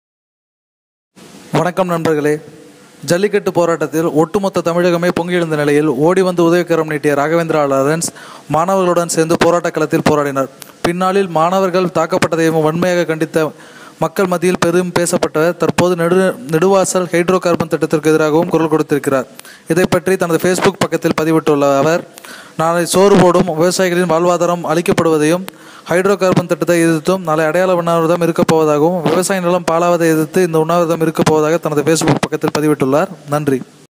வாடிவாசலை அடுத்து நெடுவாசலுக்காக குரல் கொடுக்கும் ராகவேந்திரா லாரன்ஸ்
நெடுவாசலுக்காக உண்ணா விரதம் இருக்கப்போகும்  ராகவேந்திரா லாரன்ஸ்